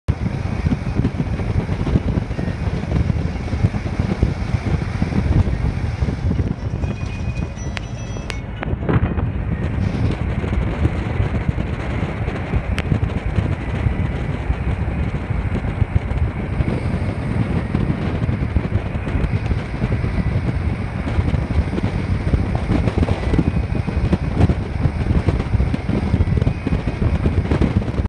Travelling Through Fikkal Illam || Fikkal Illam Nepal
Beatiful Fikkal Illam Nepal. It is famous for tea growing and horse riding